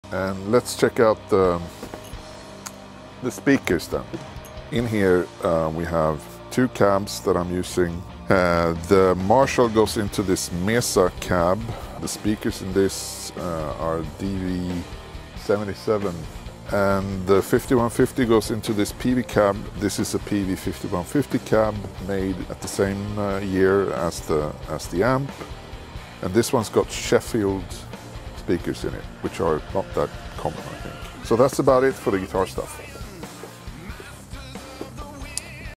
GRAND MAGUS - Studio report, part 5 (SHORTS)
Metal Brethren! ⚔️ As we delve deeper into the heart of the sonic forge, the saga of Grand Magus' sonic conquest continues ✨ Prepare to witness the amplification of metal mastery that features on our forthcoming opus.